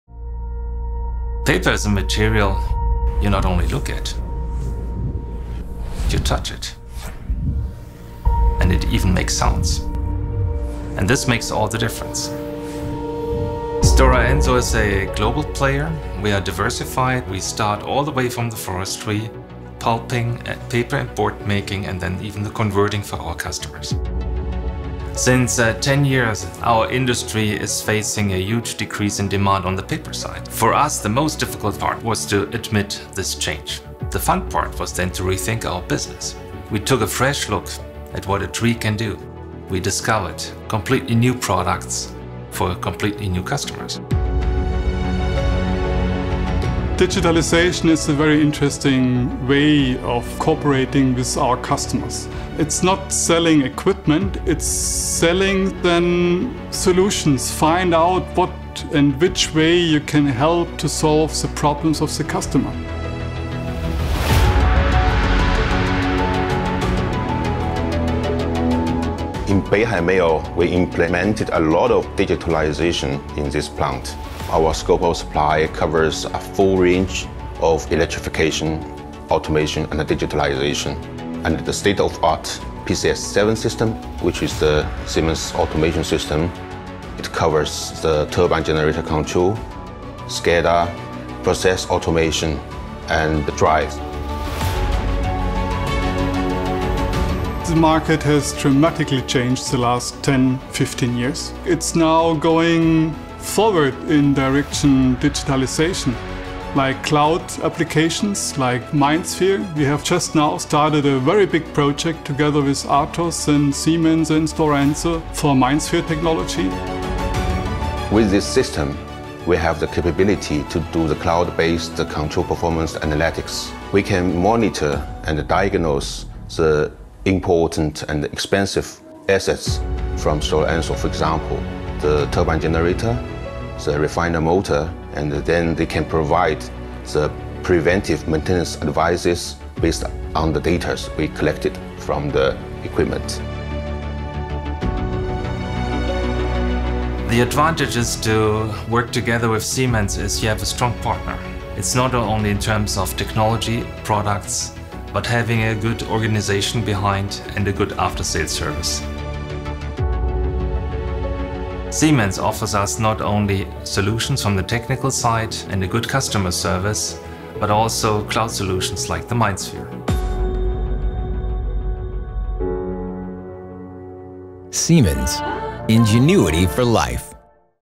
Fiber Industry of the future - Stora Enso Tiger Project, Beihai, China
Embracing disruption in the pulp and paper industry, Stora Enso dares to do a major investment and, in partnering up with Siemens, builds a digital factory from the ground up. Aiming at the Chinese growth market, the new Greenfield board mill in Beihai is a milestone for digitalization and customer centricity in the fiber industry.